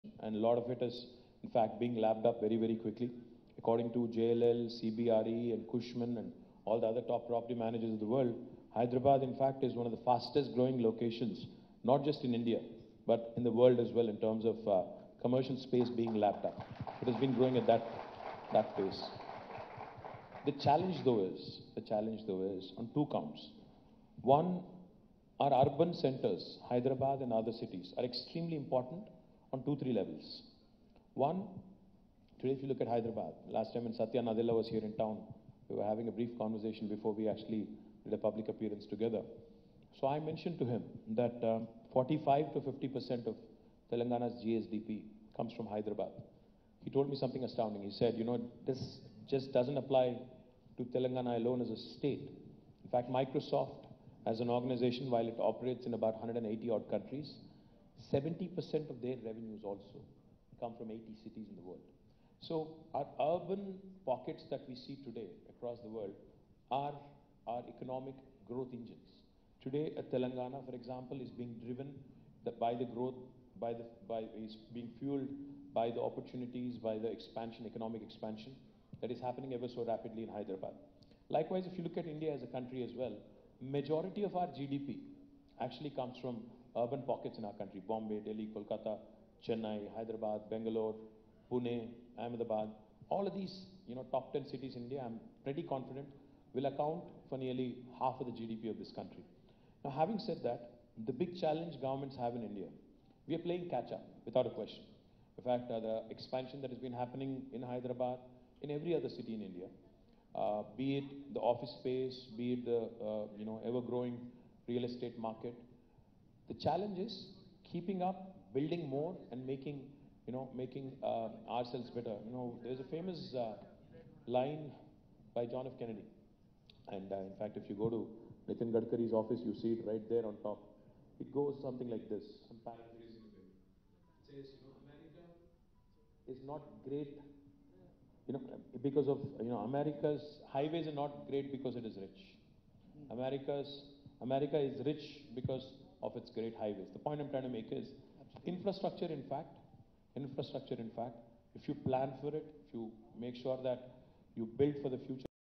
45% to 50% of Telangana GSDP is From Hyderabad Says KTR | The Spirit of Hyderabad | NTV
45% to 50% of Telangana GSDP is From Hyderabad Says KTR | The Spirit of Hyderabad | NTV